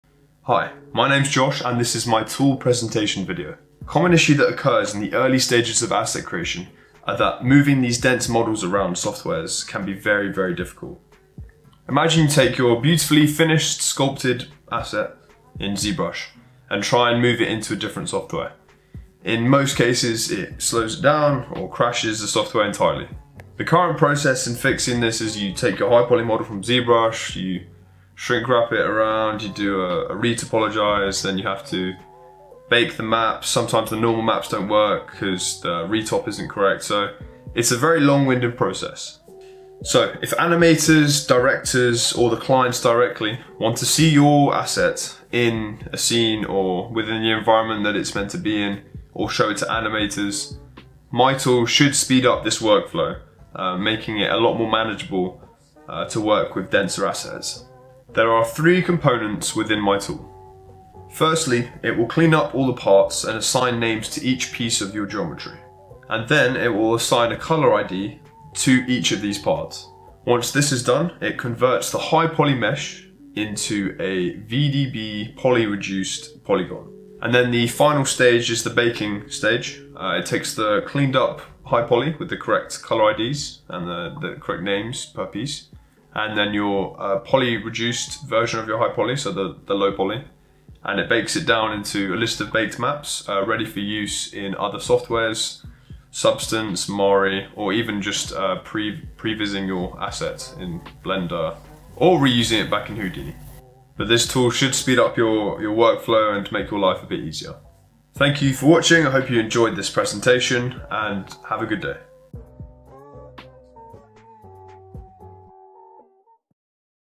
De-Dense Tool - Problem Demonstration
Short 1-2 minute Narrated video showing the problem I have identified/selected. This is linked to the tool I will make in the coming weeks.